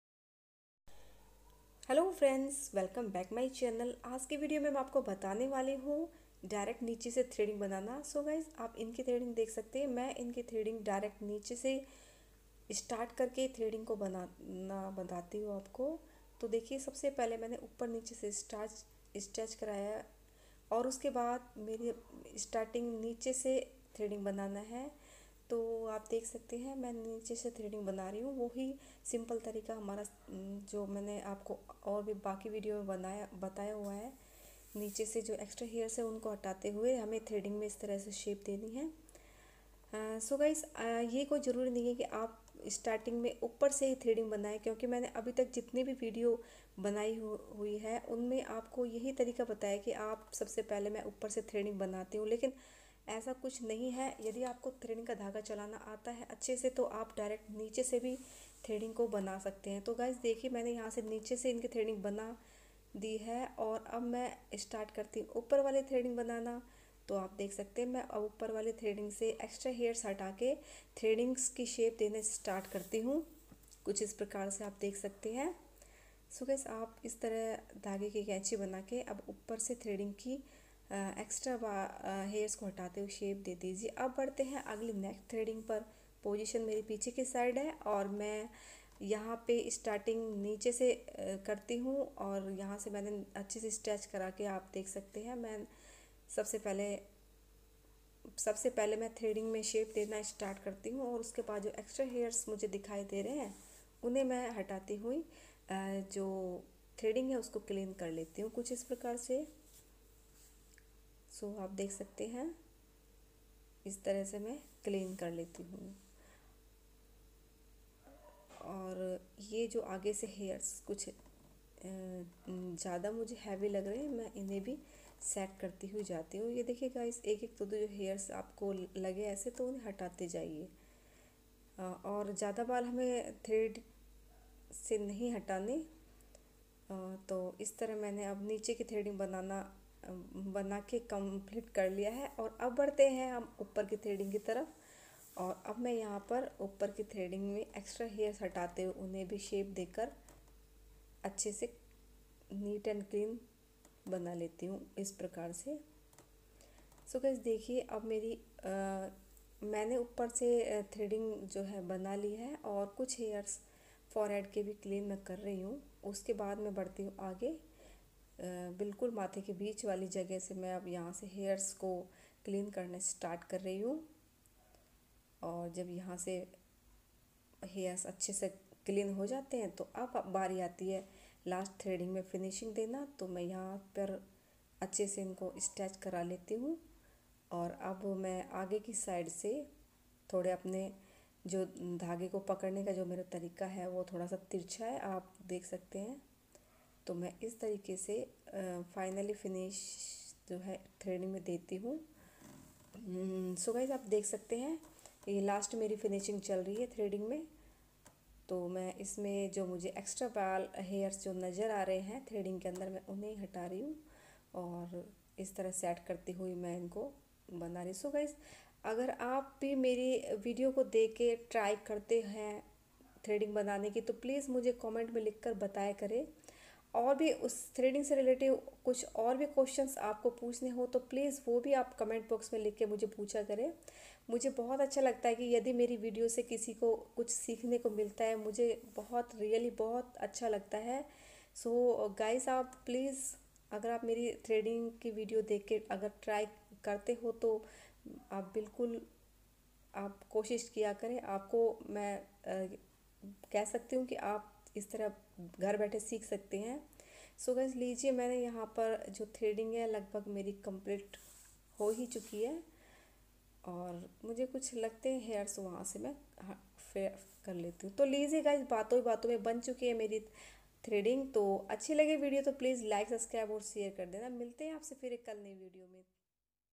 डायरेक्ट नीचे से STARTकरें Threading बनाना | simple tareka Threading bananeka |Eyebrow tutorial
My channal name#priyadeephome

Hair cutting

Hello guys kaise hain aap Sabhi
I hope aap acche Honge aaj ki video mein main direct Niche Se thrading banana bataya hai I hope video ko Pasand Aaye To please like kar dena.

Threading eyebrow. Bina Dhage se eyebrow).  Eyebrow banane ka tarika. Niche Sahiba Kaise banaen. Simple easy tarika.